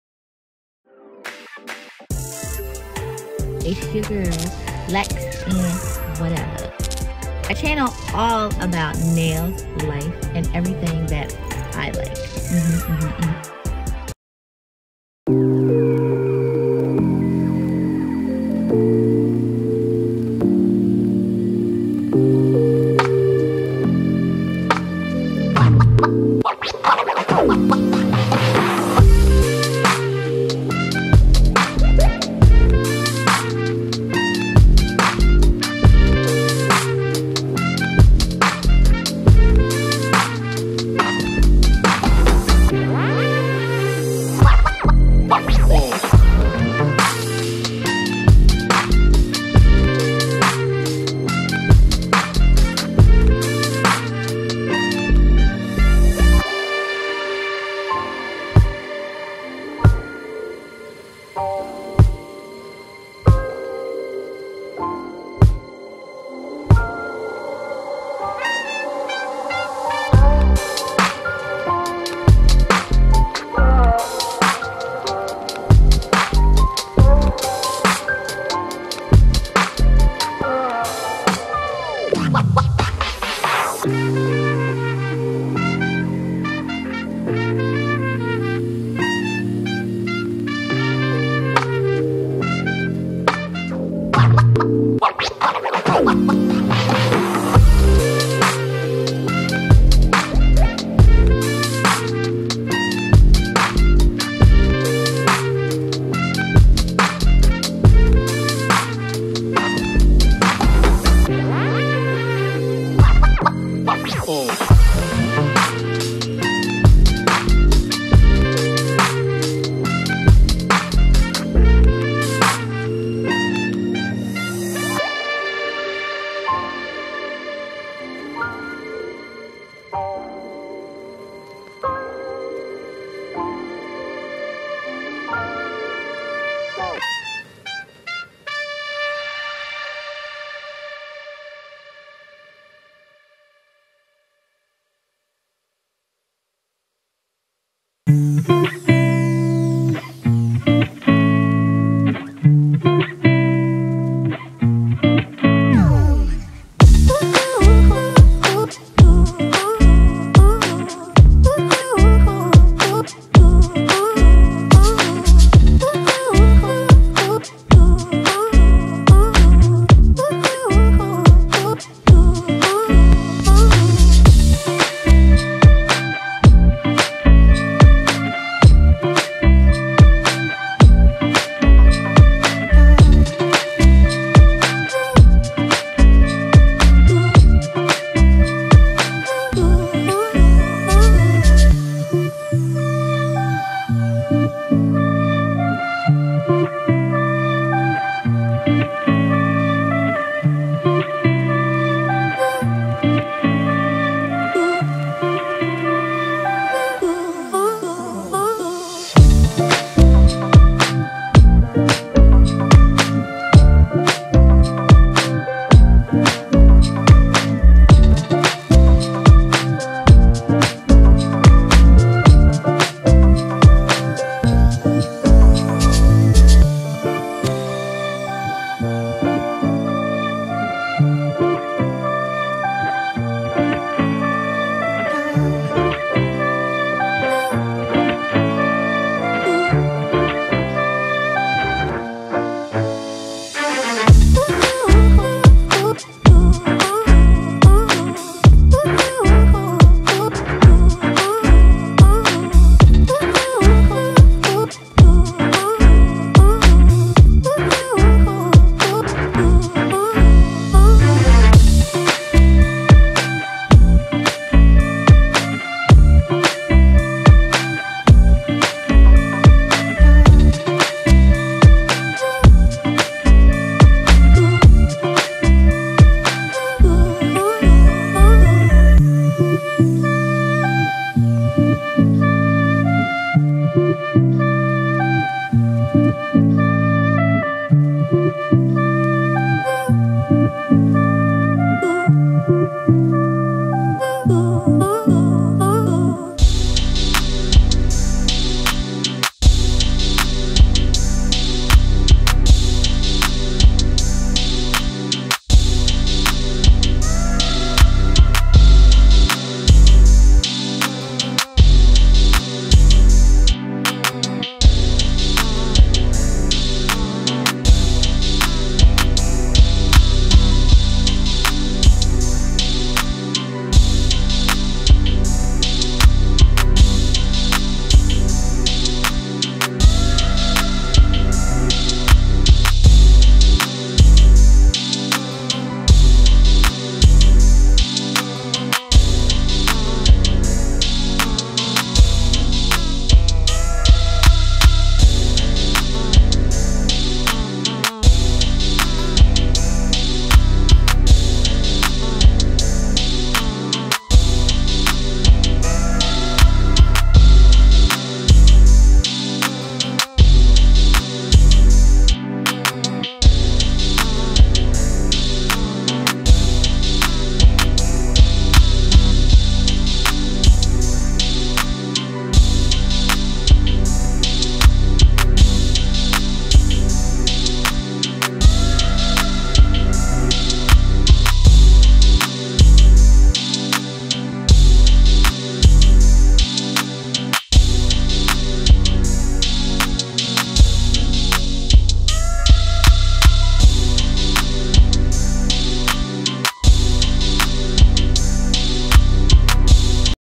KISS VELOUR FANTASY Press On Nails
Girl, are you ready for Valentines Day? It is not too late!
Press On Nails are a life saver

I did not think I would like these simply because they were so short! But I loved how they turned out after I shaped them more square. Don't be discouraged by the shape, you can file them to make them look realistic.